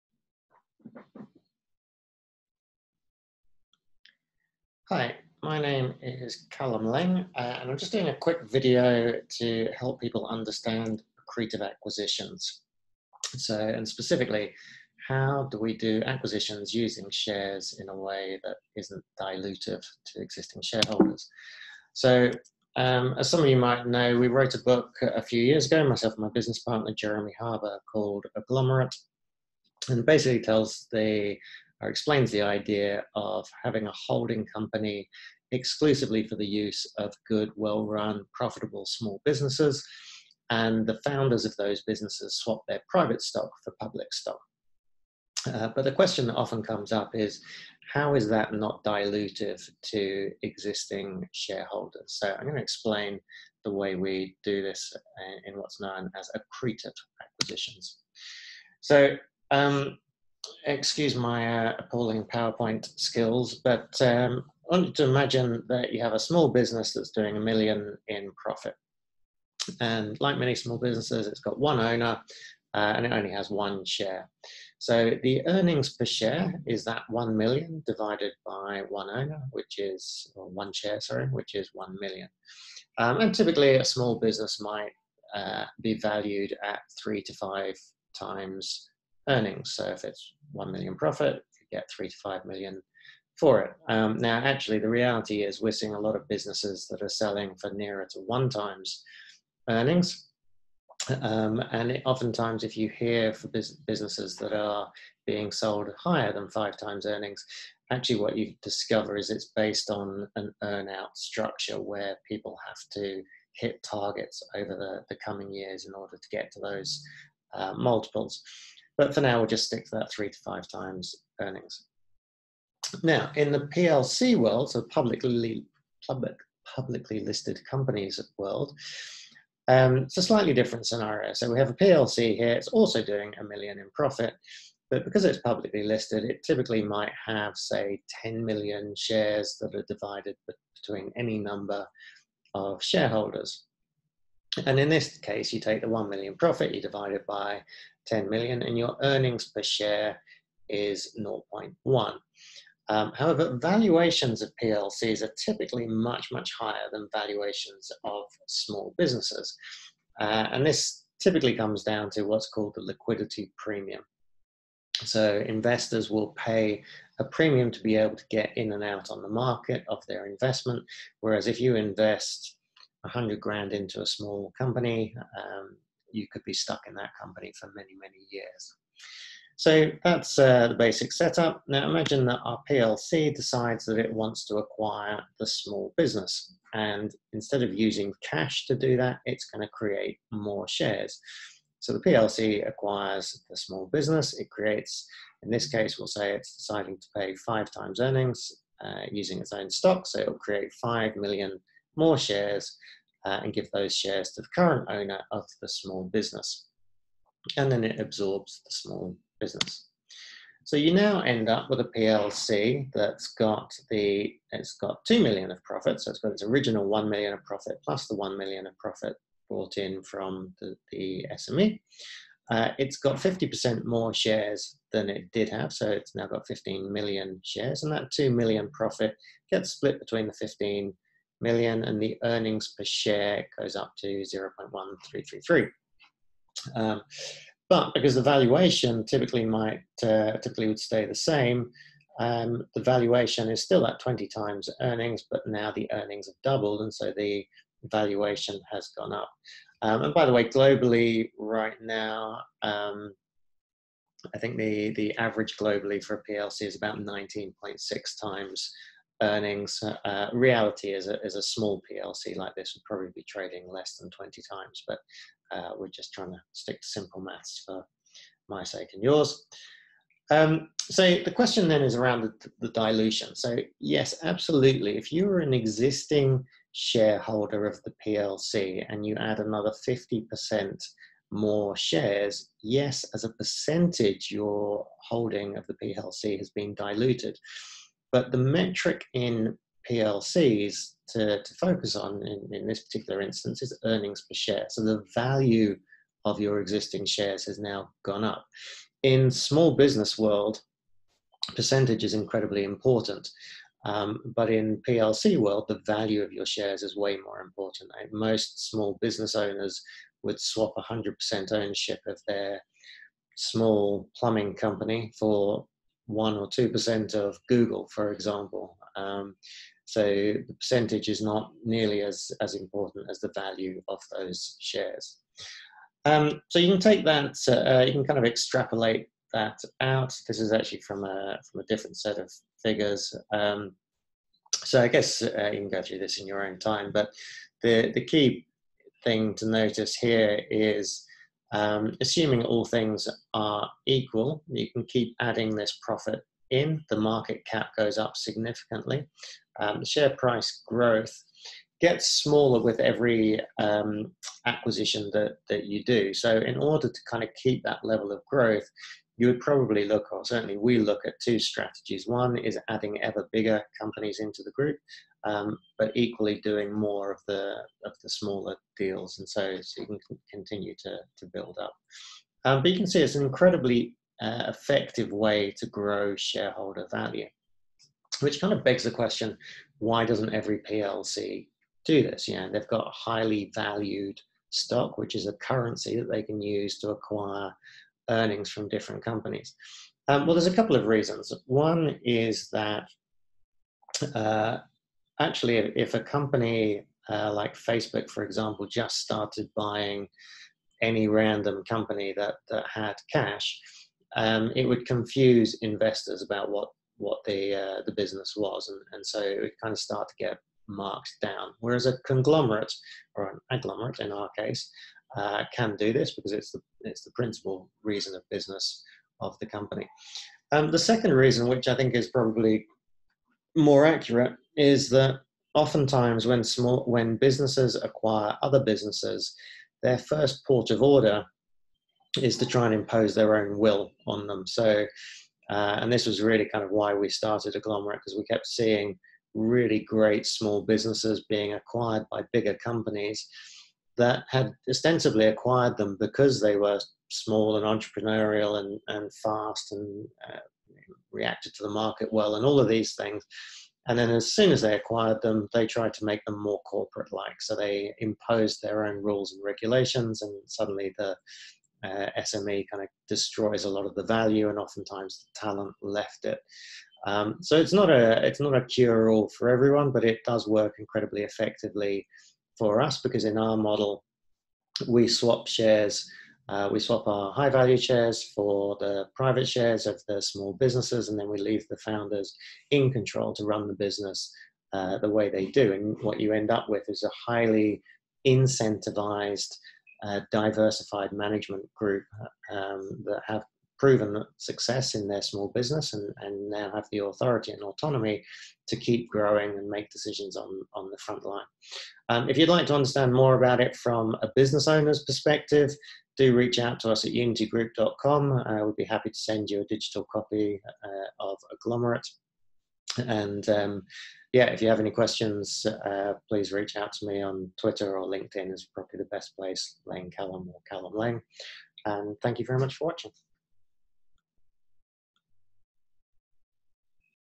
Accretive Acquisitions - Understanding the fundamentals
Using shares to acquire companies, and EBIT, is dilutive in percentage terms but by using Earnings Per Share (EPS) accretive acquisitions the value of each share rises with every acquisition.